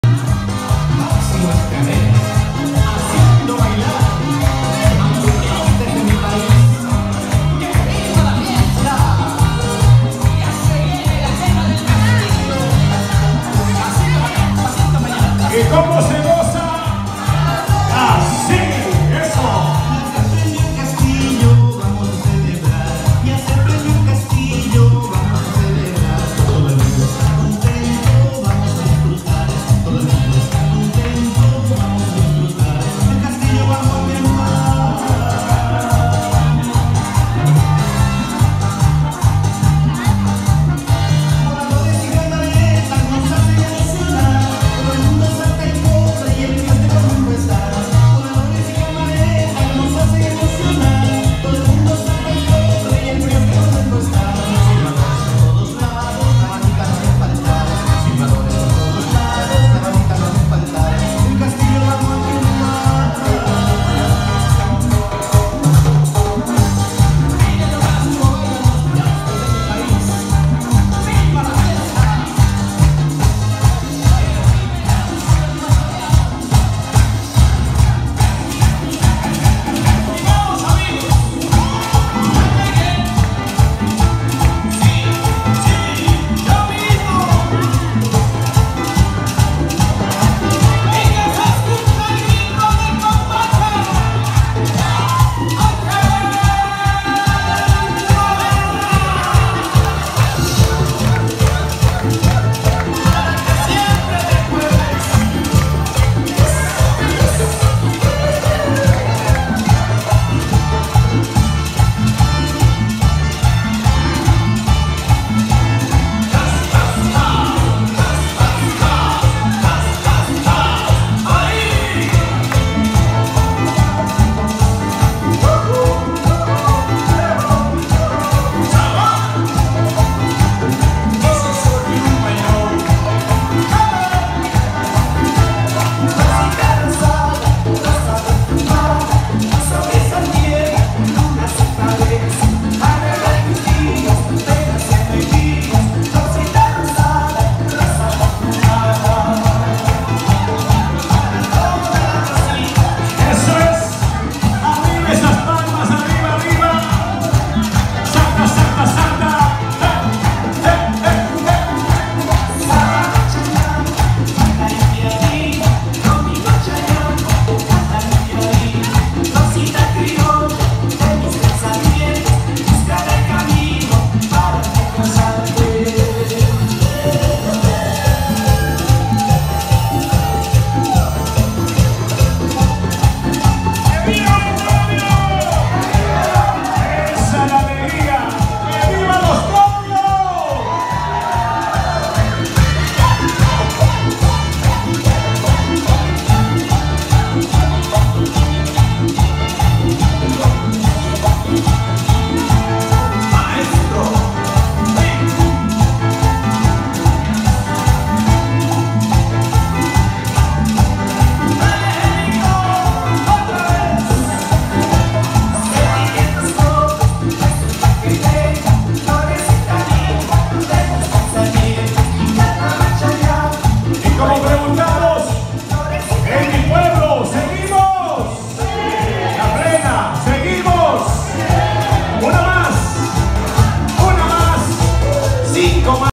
Scorpion Eventos Boda de Luis & Emma. Staten Island NY Julio 2021
Scorpion Eventos en la Boda de Luis & Emma animacion Manolo Pazmiño. Fotógrafo Braulio Arevalo. Camarógrafo Ecuamex Vídeo Film. Staten Island NY Julio 2021